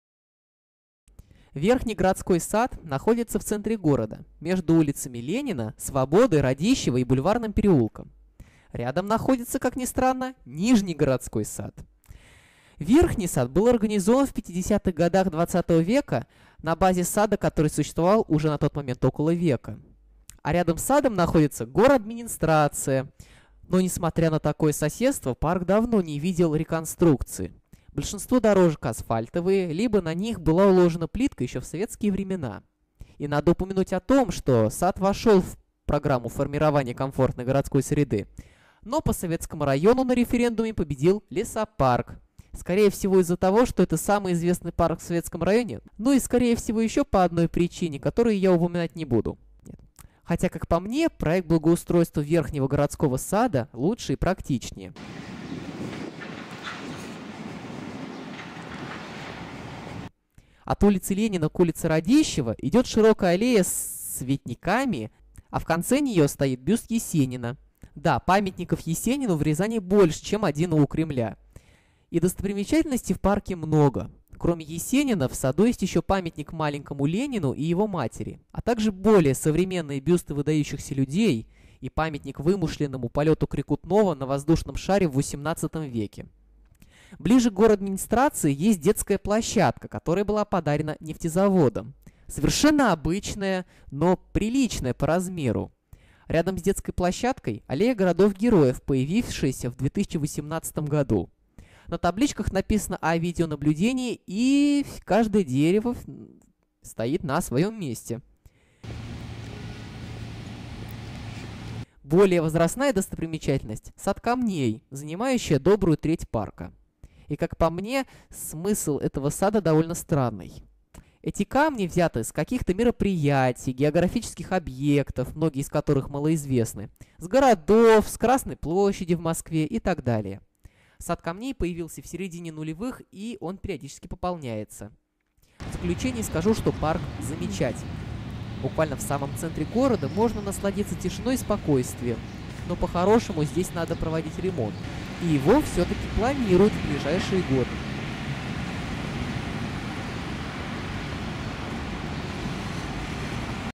Верхний городской сад. Тишина и зелень в самом центре Рязани (Парки Рязани 1)
Это - новый проект "Парки Рязани", в котором мы будем гулять по паркам города.
В первом выпуске мы посетим Верхний городской сад, который располагается в самом центре города.